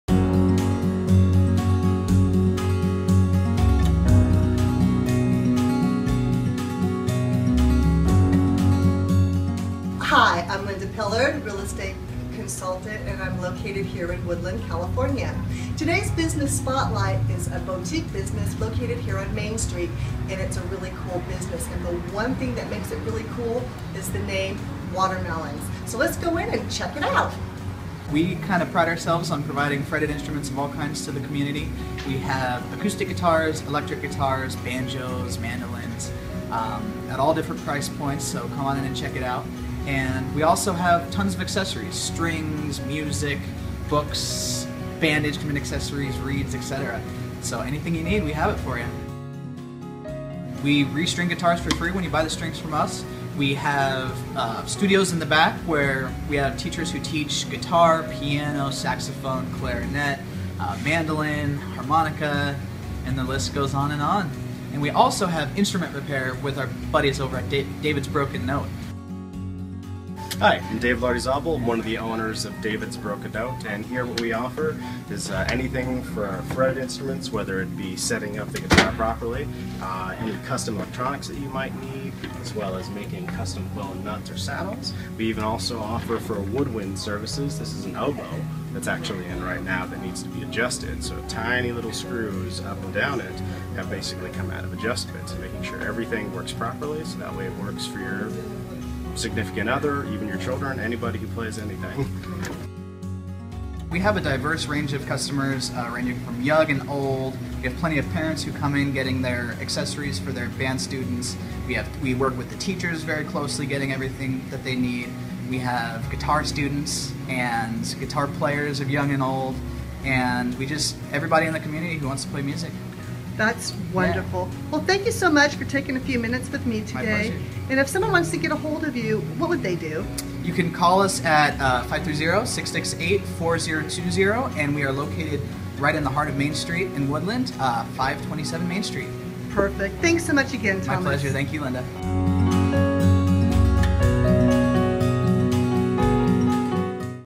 Watermelon Music located in Davis - Music Store
A wonderful music store with great customer care that is NOW LOCATED IN DAVIS Linda Pillard Business Spotlight video is a short video about the business.  Click the link and Check it out!